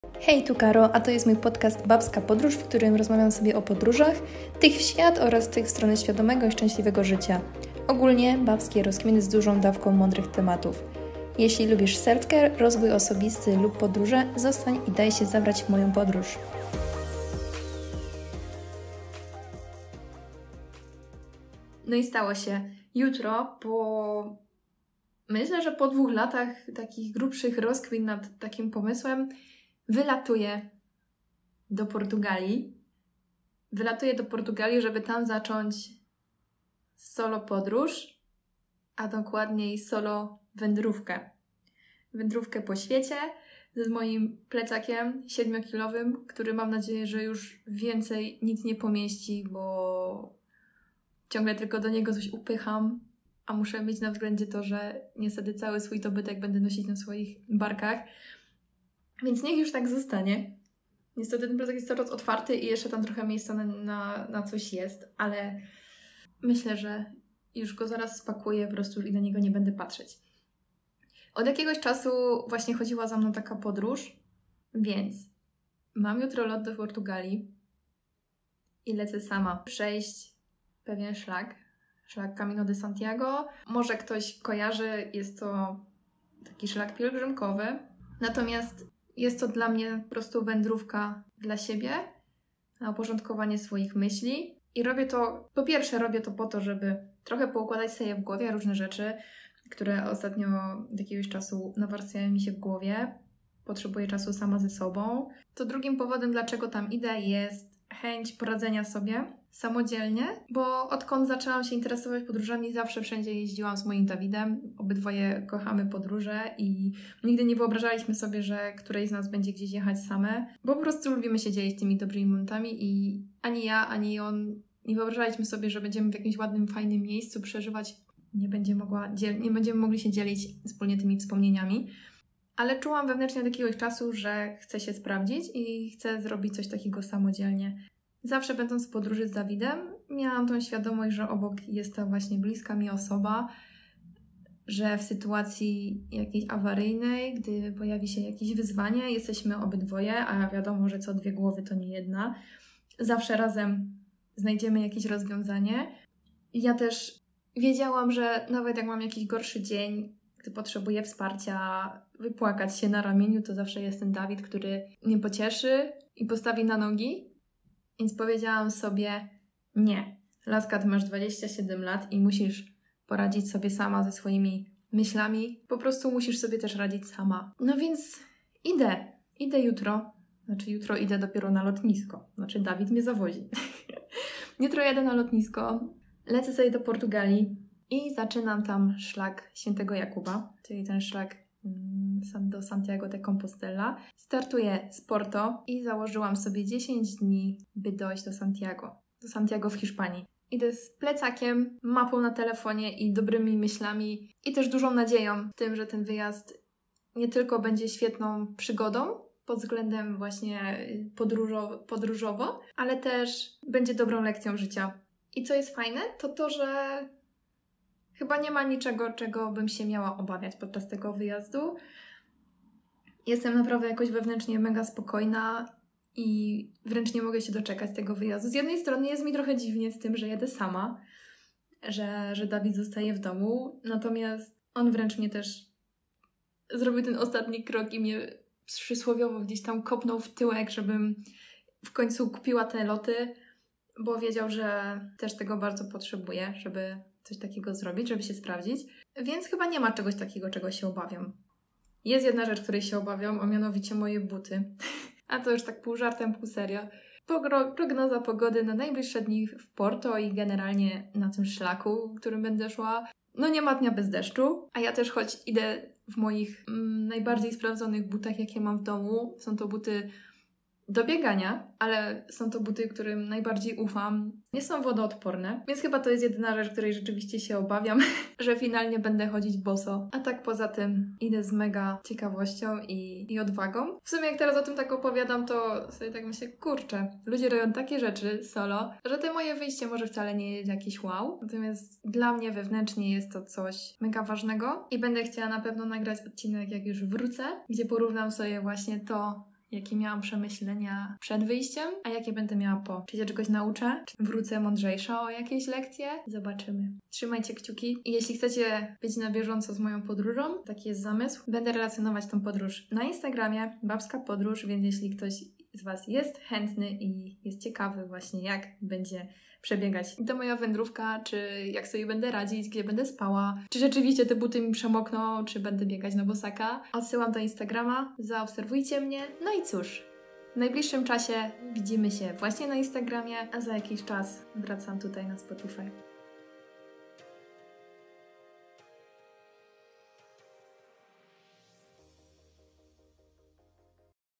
#4 moja pierwsza solo podróż, idę sama z Portugalii do Hiszpanii!
Dzisiaj krótko, jako mała zajawka podróży, w jaką idę - sama! Za parę dni wyruszam w wędrówkę z Porto do Santiago de Compostella, tylko ja i mój 7-kilogramowy plecak. Przede mną do pokonania około 225km oraz swoje słabości. W tym odcinku dowiesz się:

- dlaczego zdecydowałam się na solo podróż?

- kiedy jadę i ile czasu planuję na swoją wędrówkę?

- co na to Dawid?

- czego się najbardziej obawiam?

miłego słuchania! :)

Jeśli chcesz wesprzeć moją pracę, możesz postawić mi "kawkę" :  buycoffee.to/ babskapodroz